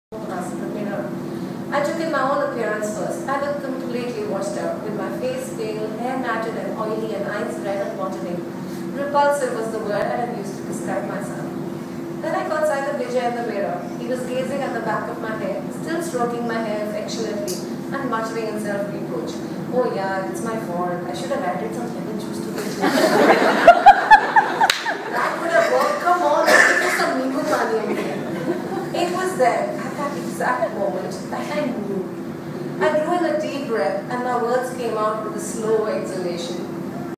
Just Married, Please Excuse Book launch - 4
Anuja Chauhan reads an extract from Yashodhara Lal's book. Like Swapan, she manages to get the crowd laughing.  Unfortunately, we didn't get the end of this one because Vijay's iPhone ran out of space (Dammit!)